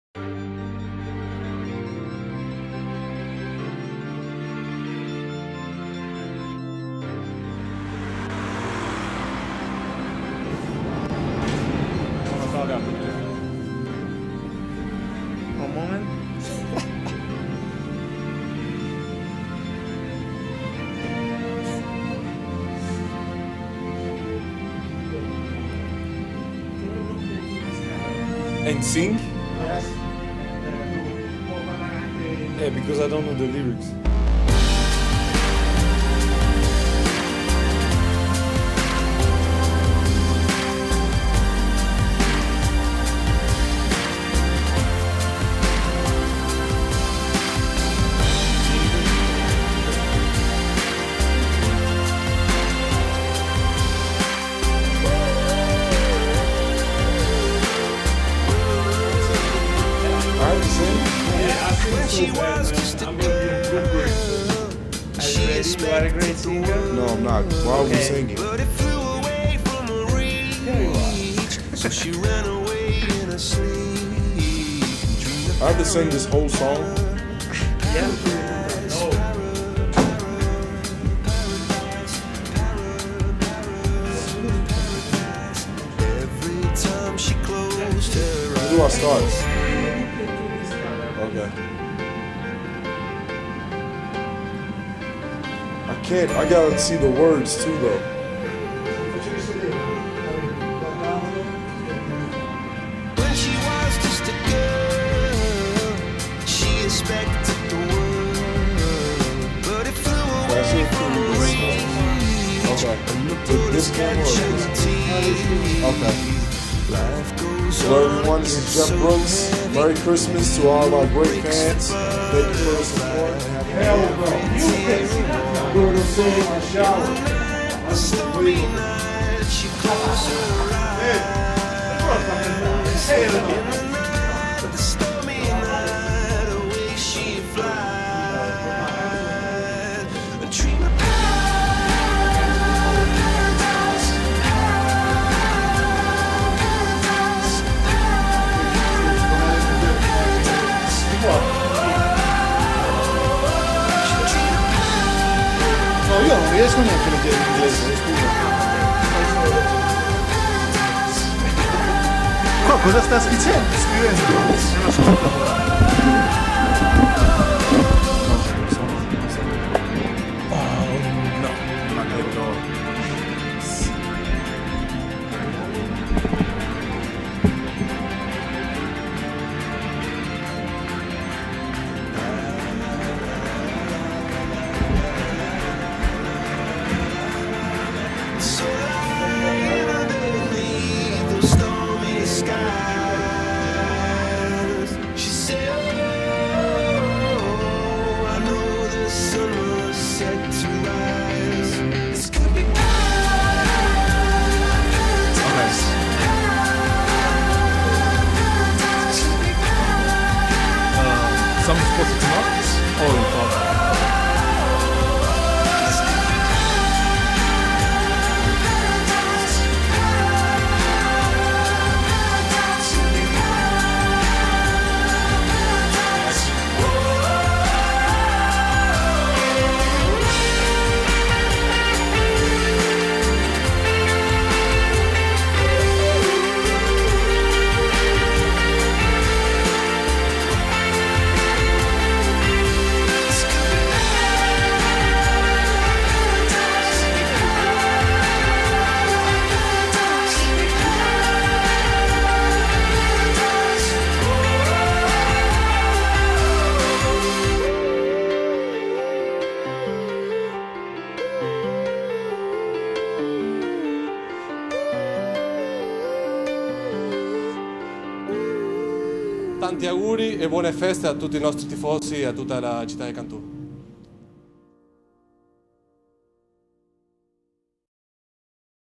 backstage video natalizio pallacanestro cantù.
backstage del video natalizio della pallacanestro cantù
di walter gorini buon natale !